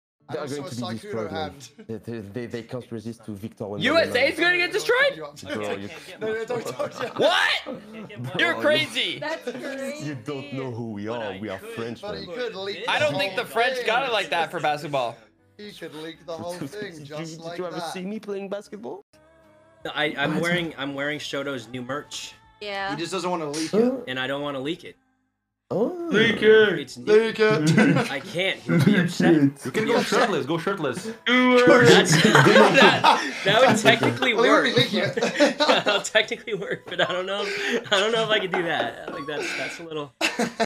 Sykkuno (almost) leaking Shoto's new merch LMAO
Follow Sykkuno!